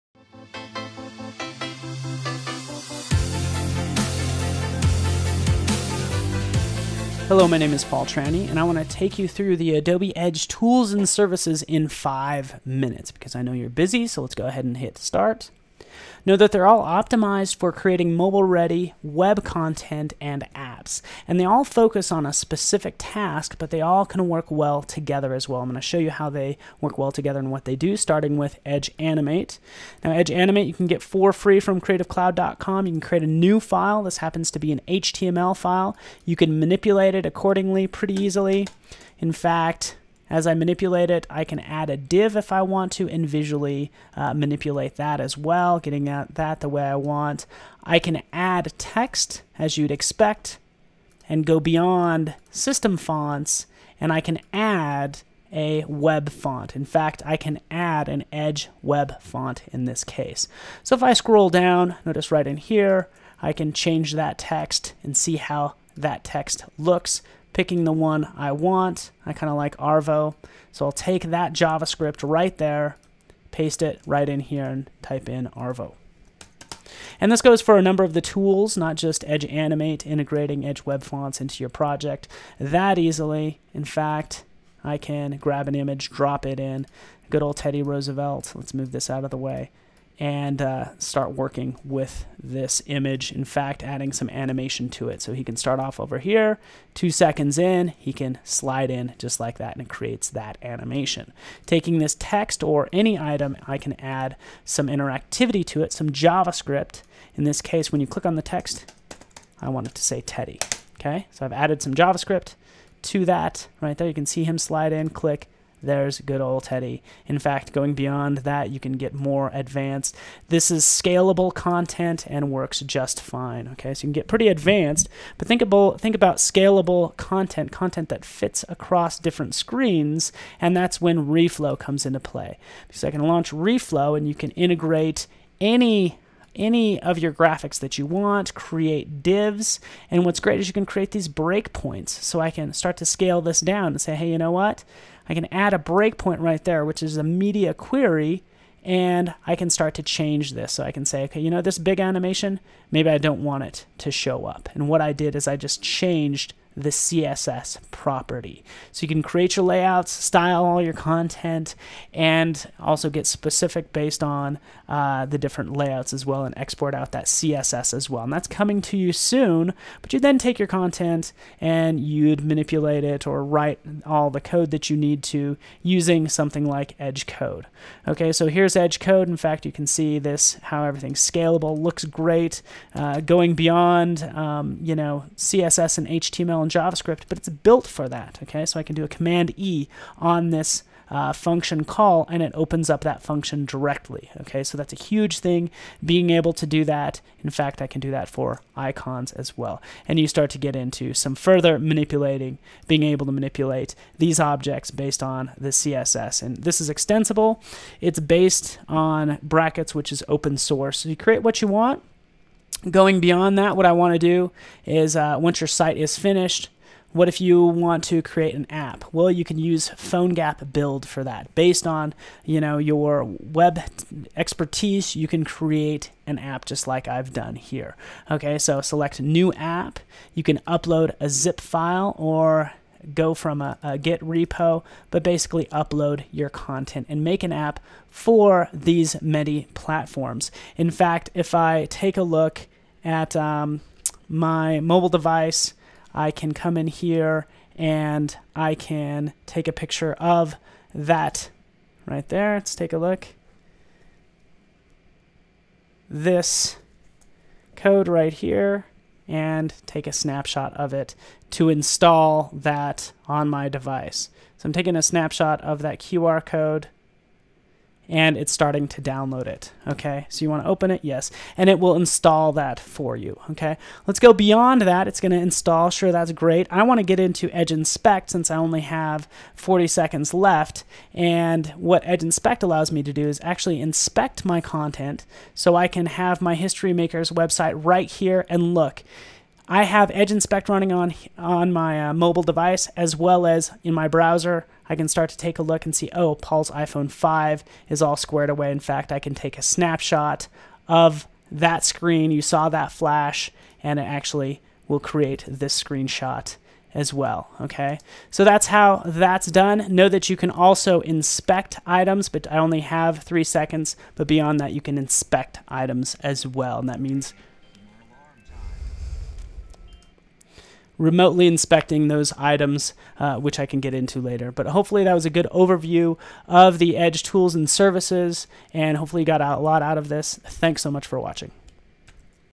Adobe Edge Tools & Services in 5 Minutes
In this video I give an overview of the Edge Tools & Services, which include:
Edge Animate
Edge Web Fonts
Edge Reflow sneak
Edge Code Preview
Edge Inspect
PhoneGap Build